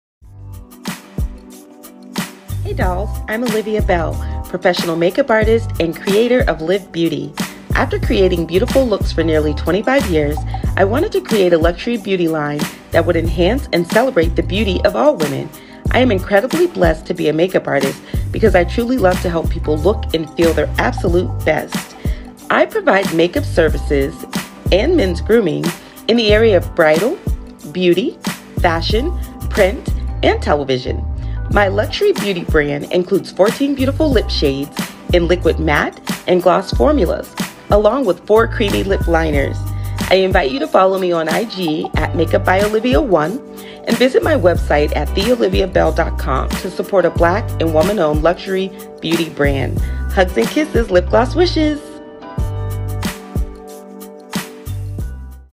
EP 5 of Black History Month -Highlighting Black Business Owners-The Olivia Bell
I wanted to take the time to shout out a handful of artist, entrepreneurs & business owners in honor of these last few days of Black History Month.

Liv by Olivia Bell

Like the song?